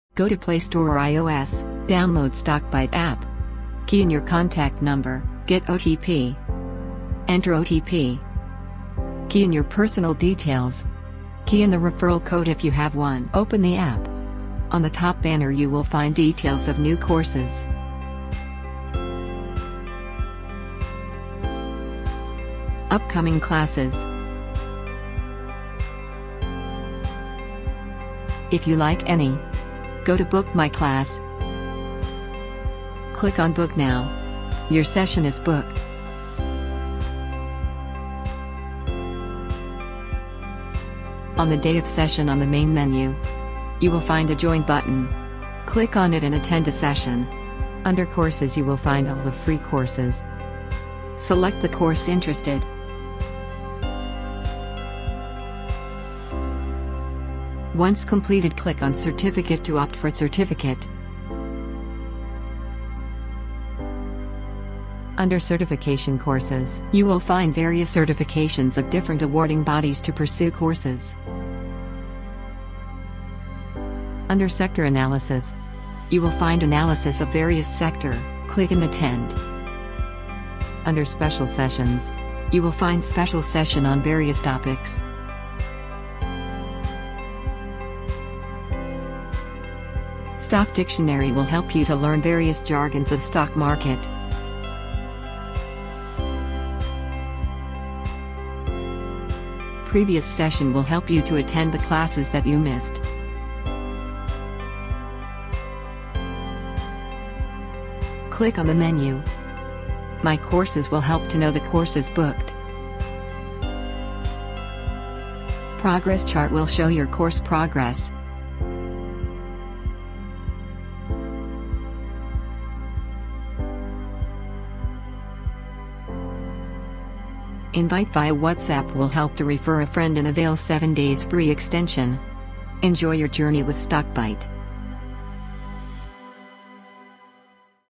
Stockbyte App Self Help Video
Watch the video for a walk through to the app and know how to use it the best and know about all the features.